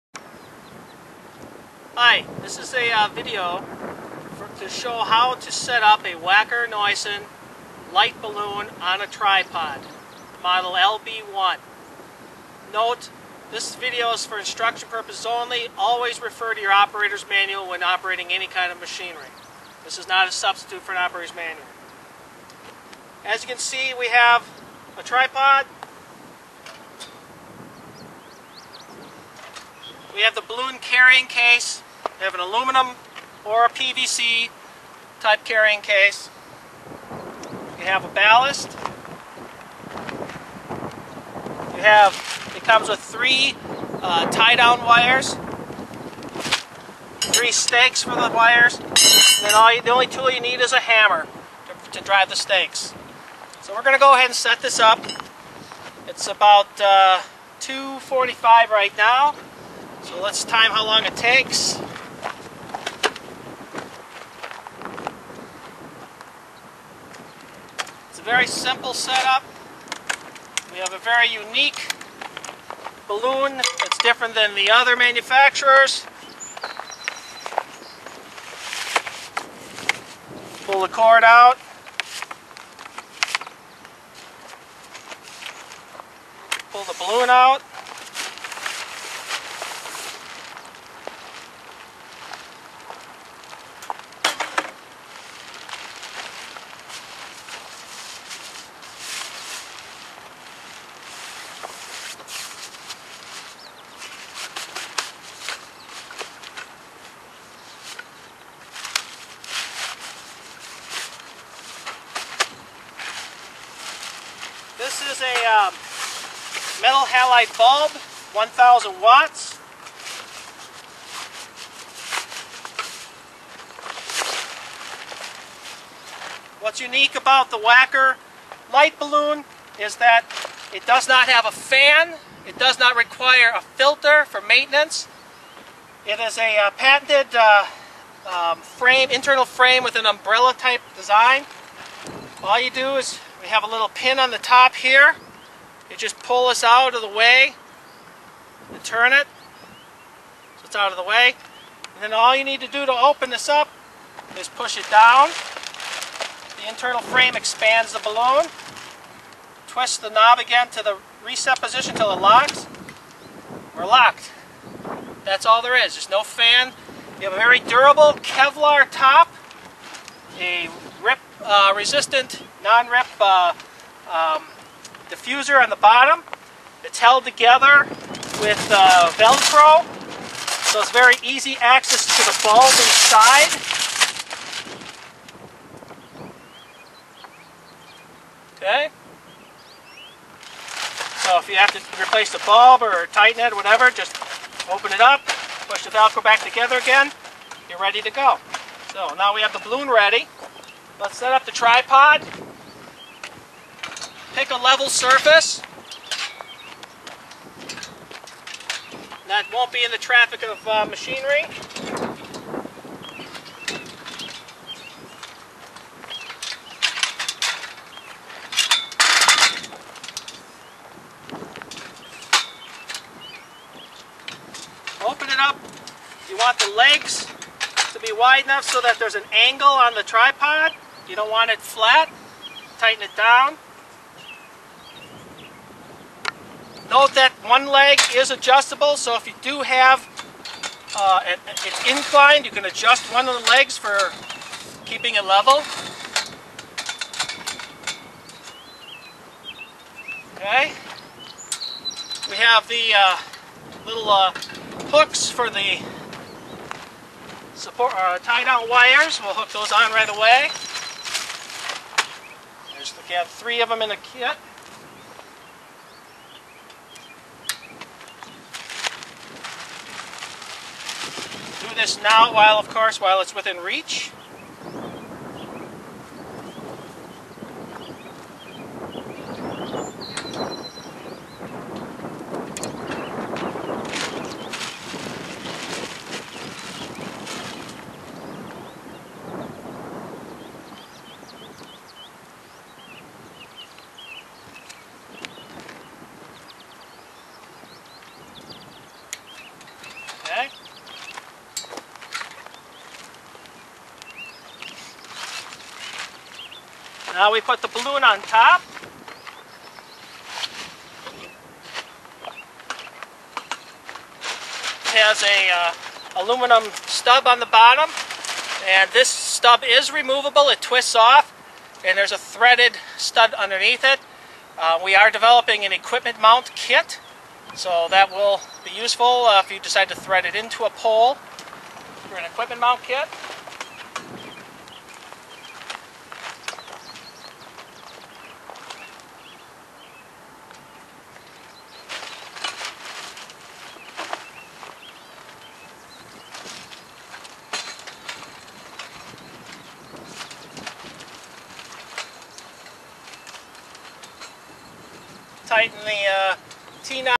Wacker Neuson Light Balloon Setup Video #1
This video explains how to set up a Wacker Neuson LB1 Light Balloon on Tripod